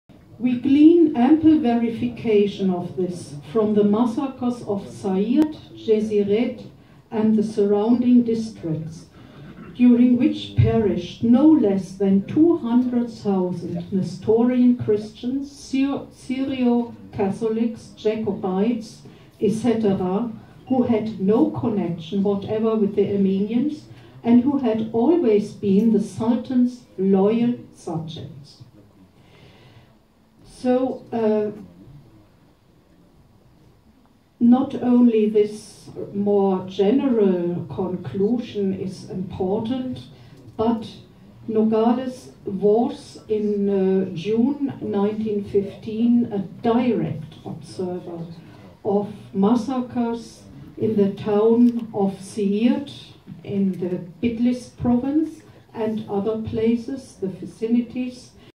Arameeërs werden afgeslacht in 1915
Arameeërs/Jacobieten werden afgeslacht ondanks dat ze loyaal waren aan de (Ottomaanse) sultan. Genocide onderzoeker dr. Tessa Hofmann.